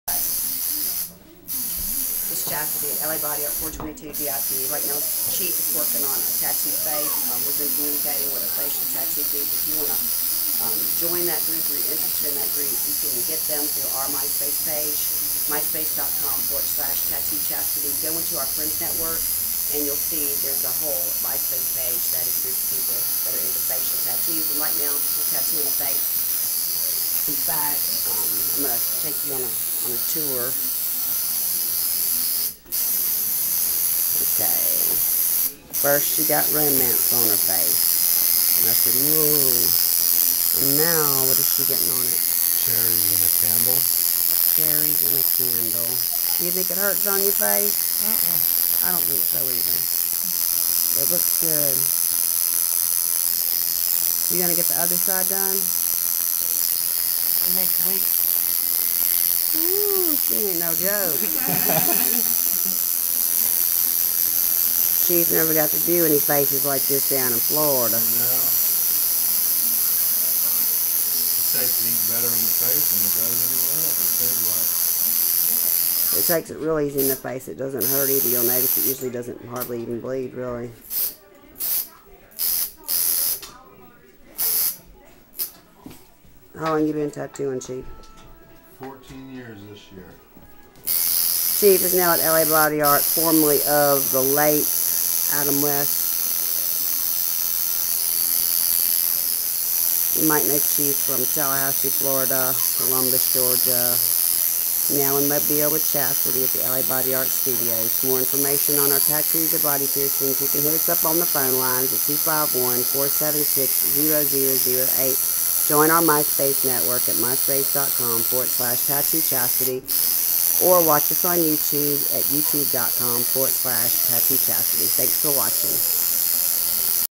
LA BODY ART FACIAL TATTOO BY CHEIF
CHEIF TATTOOS A REGULAR CUSTOMERS FACE WITH ,THE WORDS ROMANCE" AND A PAIR OF CHERRIES AND  A CANDLE..DESIGN.422 D.I.P. MOBILE, AL 423-0854